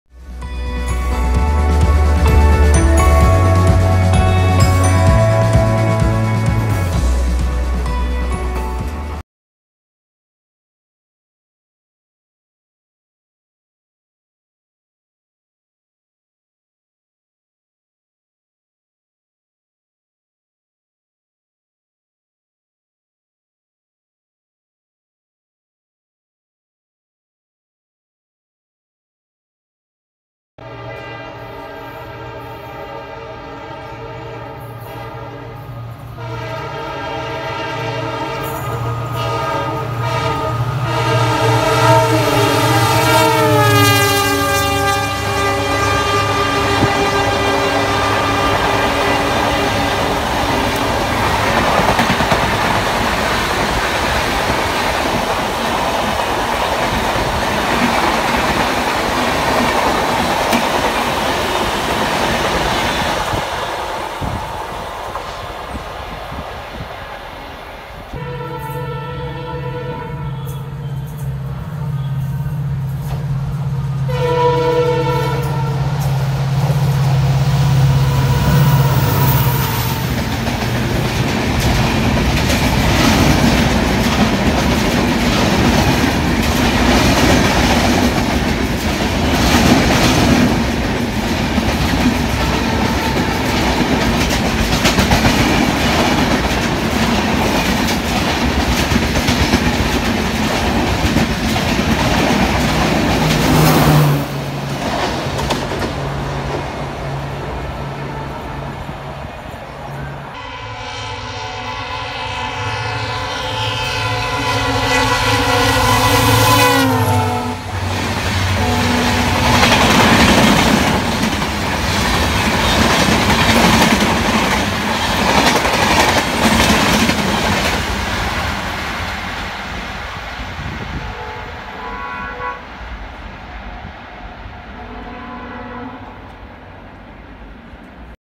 बाज़ 🦅 रंगबिरंगे EMD इंजन : NFR Trains | Rajdhani & Superfast Express - Colourful LHF EMDs
Featuring LHF Colourful EMDs With Top Class Diesel Hauled Trains.
Bonus - 20013 BAAZ 🦅 Livered Siligudi WDP 4.

JAI HIND.